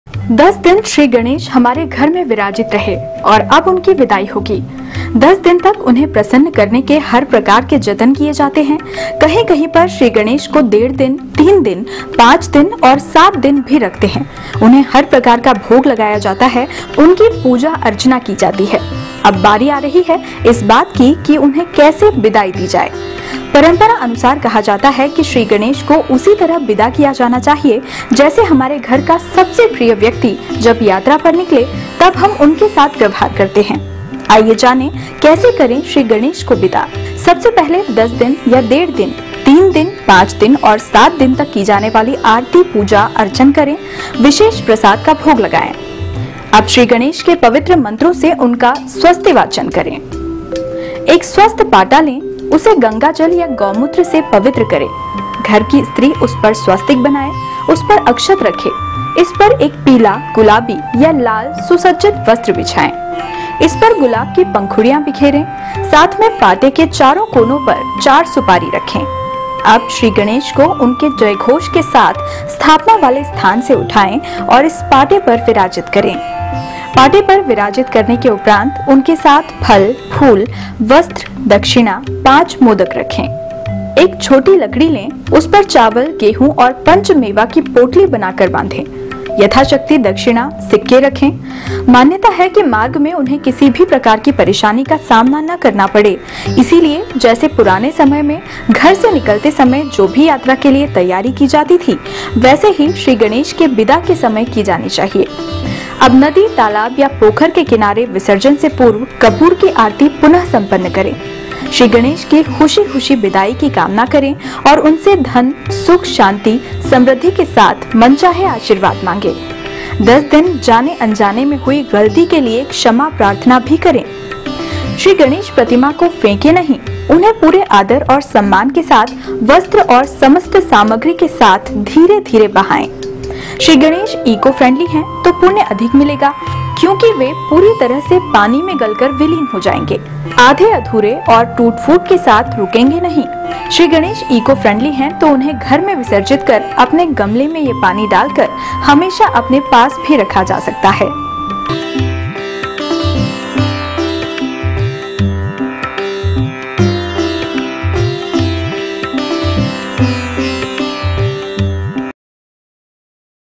श्री गणेश की कैसे हो बिदाई, जानिए 11 पौराणिक बातें
गणेश विसर्जन के यह 11 नियम आपको पता है?
आइए जानें विसर्जन के लिए कुछ ऐसी छोटी-छोटी काम की बातें, जो हमारी परंपरा में हमने अपने बड़ों से सुनी है।